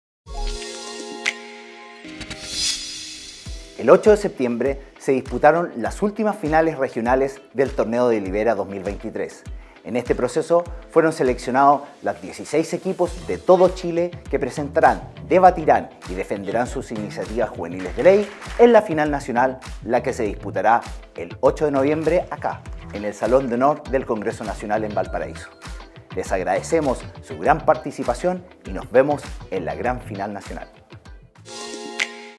Torneo Delibera 2023 - Concluye etapa de finales regionales
Con las finales regionales de Valparaíso y Maule, el pasado viernes 8 de septiembre fueron elegidos los últimos dos equipos que disputarán la gran final del Torneo Delibera 2023, iniciativa de formación cívica impulsada por la Biblioteca del Congreso Nacional de Chile desde hace más de 12 años.